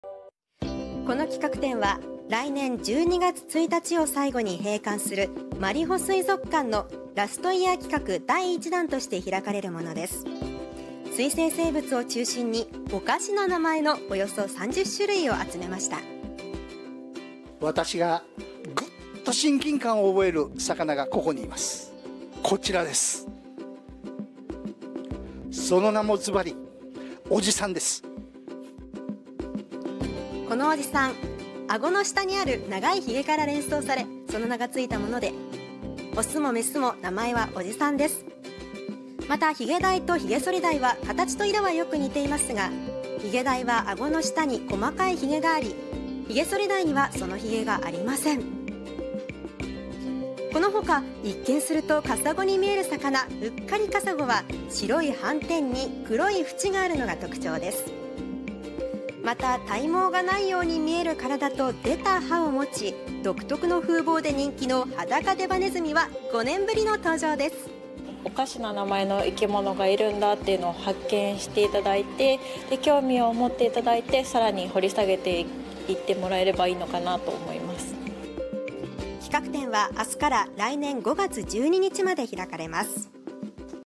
【広島を楽しもう！】「おじさん」「ヒゲソリダイ」”おかしな名前のいきもの展” １５日から開催 広島市西区・マリホ水族館
おかしな名前の生き物を集めた企画展があす１５日から広島市西区のマリホ水族館で開かれます。

この企画展は来年１２月１日を最後に閉館するマリホ水族館のラストイヤー企画第１弾として開かれるものです。
水生生物を中心におかしな名前のおよそ３０種類を集めました。

【浜田記者】
「私がぐっと親近感を覚える魚がここにいます。こちらです。その名もずばり、オジサンです」

このオジサン、あごの下にある長いひげから連想されその名がついたもので雄も雌も名前はオジサンです。
また、ヒゲダイとヒゲソリダイは形と色はよく似ていますが、ヒゲダイはあごの下に細かいひげがあり、ヒゲソリダイにはそのひげがありません。
このほか、一見するとカサゴに見える魚、「ウッカリカサゴ」は白い斑点に黒い縁があるのが特徴です。
また体毛がないように見える体と出た歯を持ち、独特の風貌で人気のハダカデバネズミは５年ぶりの登場です。

【マリホ水族館・金丸  仁美さん】
「おかしな名前の生き物がいるんだというのを発見していただいて、興味を持っていただいてさらに掘り下げてもらえればいい」

企画展はあす１５日から来年５月１２日まで開かれます。